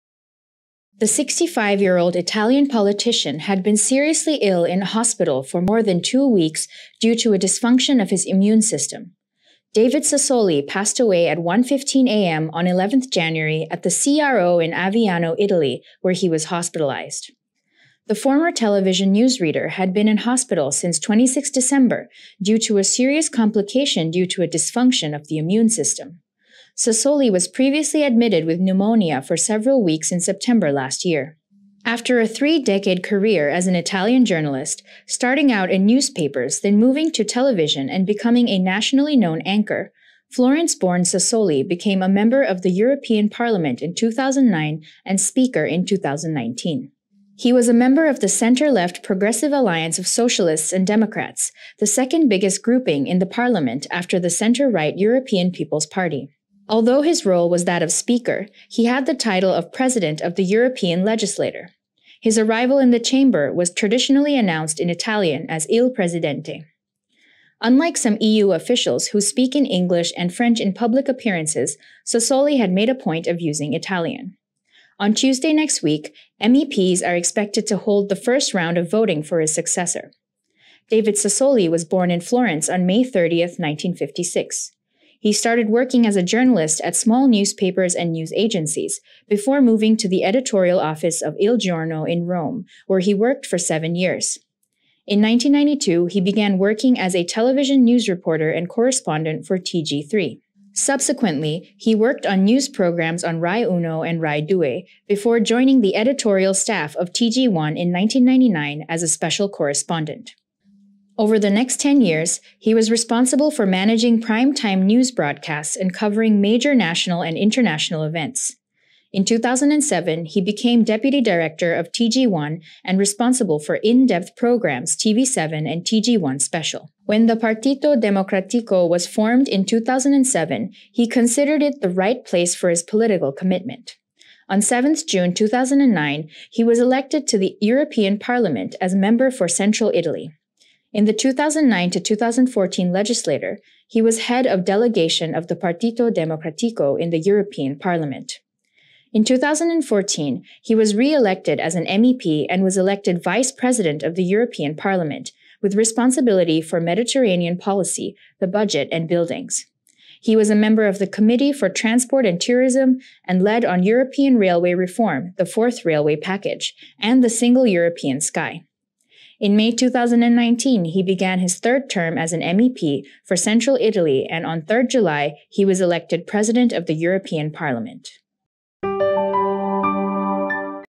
David Sassoli, the Italian Journalist Who Worked His Way Up in Politics
INTERNATIONAL: The 65-year-old Italian had been seriously ill in hospital for more than two weeks due to a dysfunction of his immune system.

“David Sassoli passed away at 1.15am on 11 January at the CRO in Aviano, Italy, where he was hospitalized,” his spokesman Roberto Cuillo has tweeted. “The date and place of the funeral will be announced in the next few hours.”

The former television newsreader had been in hospital since 26 December due to “a serious complication due to a dysfunction of the immune system.” Sassoli was previously admitted with pneumonia for several weeks in September last year.

After a three-decade career as an Italian journalist, starting out in newspapers then moving to television and becoming a nationally known anchor, Florence-born Sassoli became a member of the European Parliament in 2009, and speaker in 2019.

He was a member of the centre-left Progressive Alliance of Socialists and Democrats, the second-biggest grouping in the parliament after the centre-right European People’s party.

Although his role was that of speaker, he had the title of president of the European legislature. His arrival in the chamber was traditionally announced in Italian as “Il Presidente”.

Unlike some EU officials, who speak in English and French in public appearances, Sassoli had made a point of using Italian.

On Tuesday next week, MEPs are expected to hold the first round of voting for his successor.

David Sassoli was born in Florence on May 30, 1956.

He started working as a journalist at small newspapers and news agencies, before moving to the editorial office of Il Giorno in Rome, where he worked for seven years. In 1992, he began working as a television news reporter and correspondent for TG3.

Subsequently he worked on news programmes on Rai Uno and Rai Due, before joining the editorial staff of TG1 in 1999 as a special correspondent. Over the next 10 years, he was responsible for managing prime time news broadcasts, and covering major national and international events. In 2007, he became Deputy Director of TG1 and responsible for in-depth programmes TV7 and TG1 Special.

Since a young age, he was involved in educational associations, such as the Scouts and Catholic youth movements.

When the Partito Democratico (PD) was formed in 2007, he considered it the right place for his political commitment. On 7 June 2009, he was elected to the European Parliament as a member for central Italy. In the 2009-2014 legislature, he was head of delegation of the Partito Democratico in the European Parliament.

In 2014, he was re-elected as an MEP and was elected Vice-President of the European Parliament, with responsibility for Mediterranean Policy, the budget, and buildings. He was a member of the Committee for Transport and Tourism, and led on European railway reform (4th Railway Package), and the Single European Sky.

In May 2019, he began his third term as an MEP for central Italy and on 3 July, he was elected President of the European Parliament.